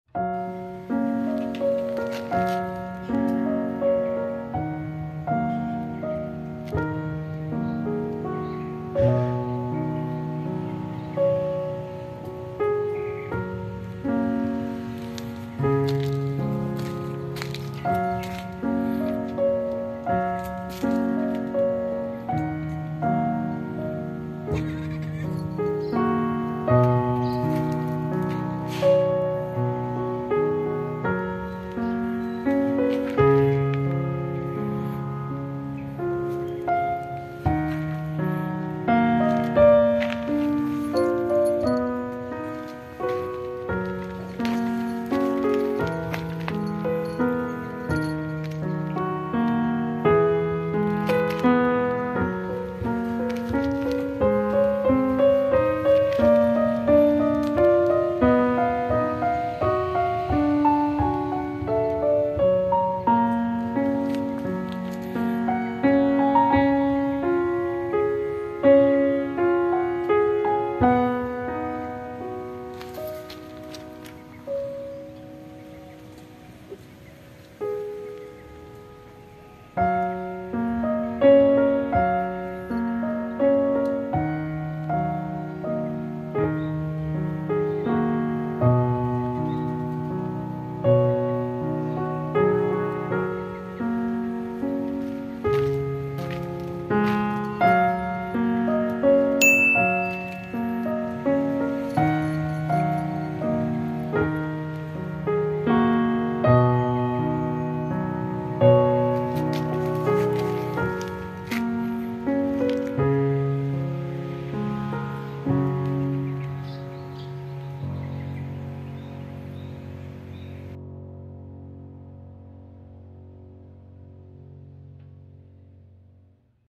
A Cute Kitten Who Is Happy When Allowed To Roam Free
Hello Cat Lovers, meet you again on Khozkha Meow Channel,

A kitten named Ash who looks very happy when allowed to roam freely outside the house.